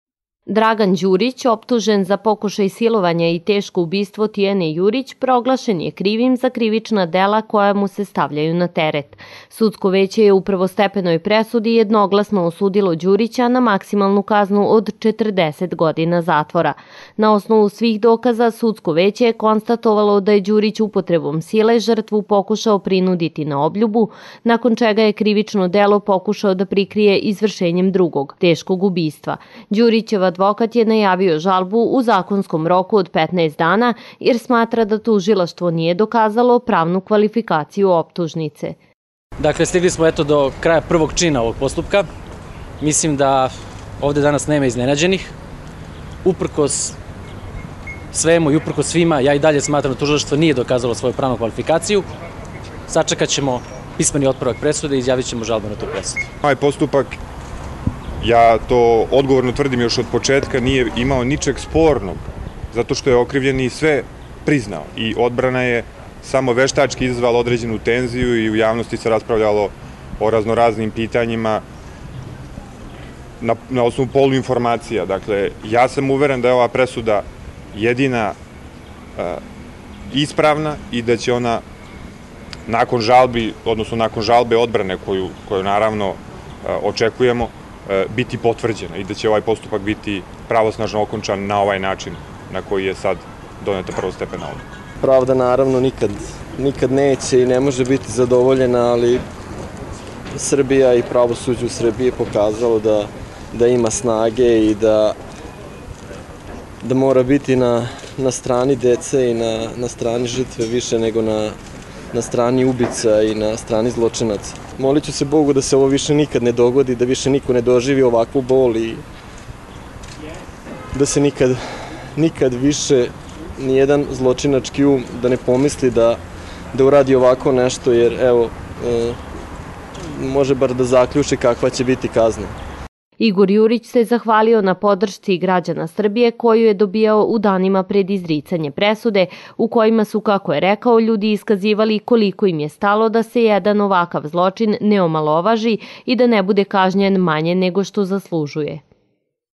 Subotica - Osuđen Dragan Đurić 16 06 2015
Viši sud u Subotici izrekao je prvostepenu presudu Draganu Đuriću od 40 godina zatvora zbog ubistva Tijane Jurić.